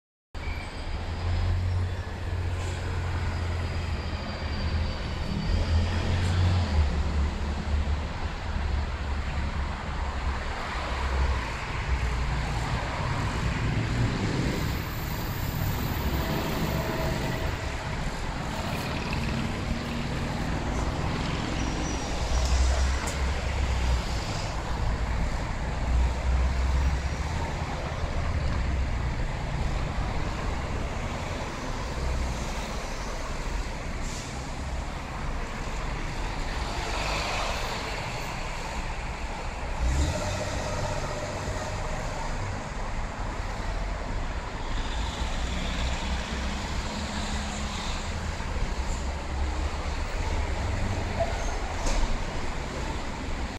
Waratah Crossing Viaduct
One of the Sydney Warratah trains crossing the viaduct near Elizabeth Street.